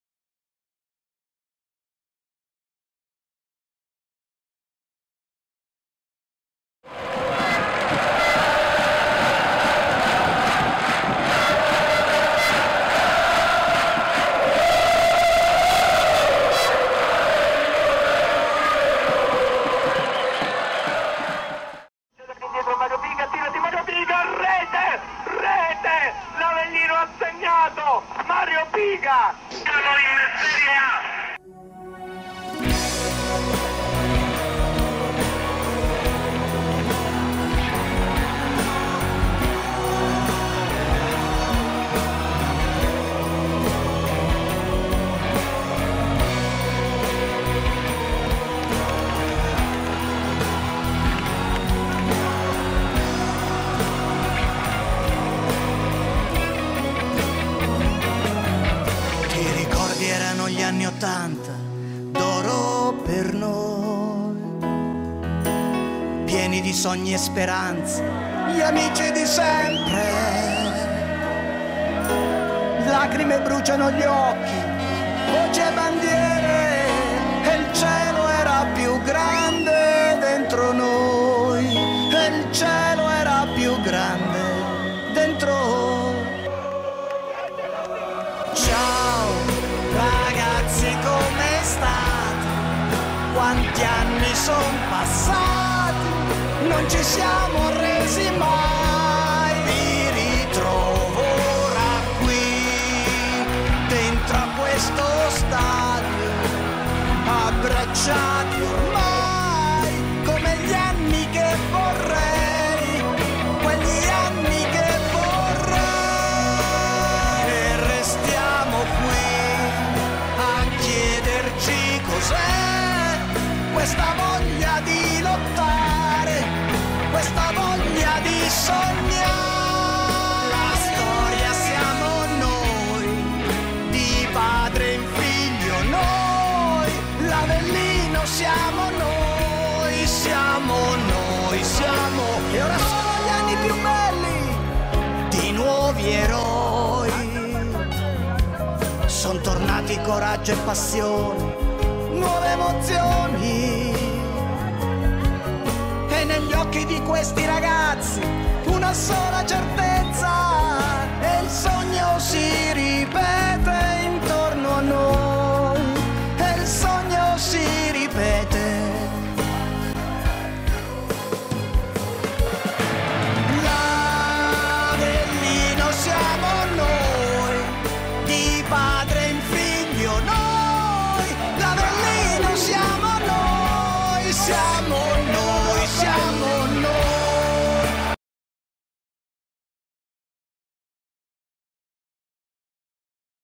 Allenamento del 26.02.14 Walter Taccone premia Peccarisi
Nel corso dell'allenamento del 26.02.14 il presidente Walter Taccone Premia Peccarisi come miglior giocatore in campo al Franco Ossola (Varese).